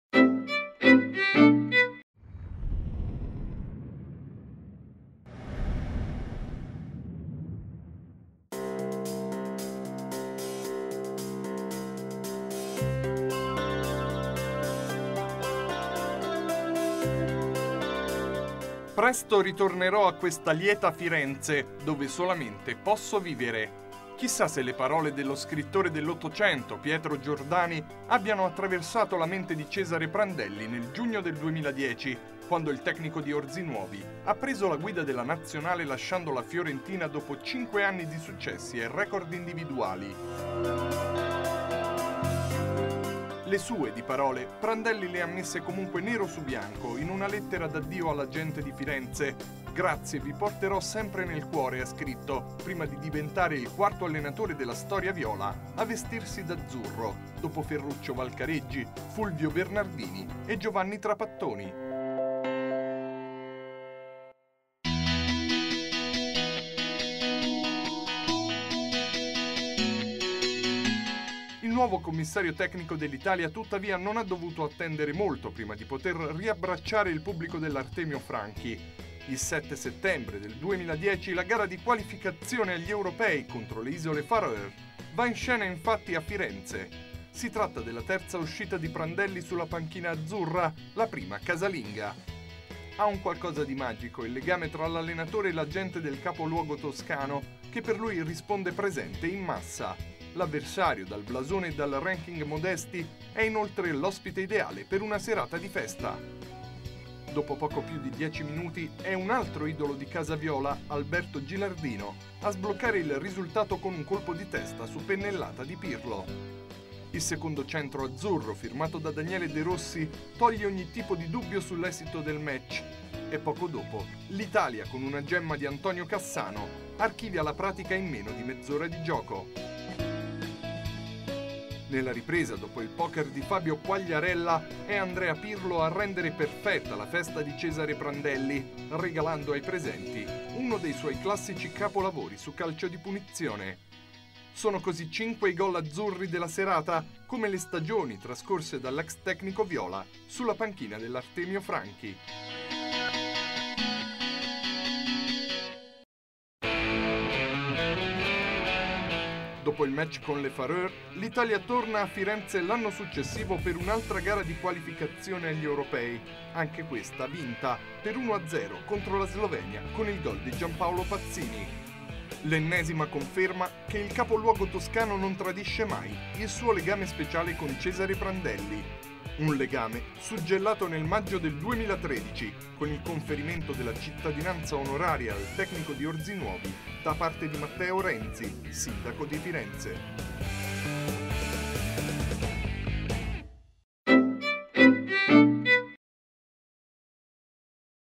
Il legame speciale tra Prandelli e Firenze - Ricordi Azzurri
Italia-Far Oer, la prima gara casalinga del Ct Azzurro si disputa nel capoluogo toscano, tanto caro all'allenatore.